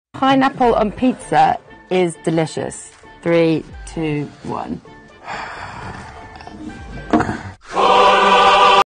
"pineapple on pizza is delicious"
Found this in one of the funniest scenes from Hayden Christensen's and Ewan McGregor's interview.

Apex video coming soon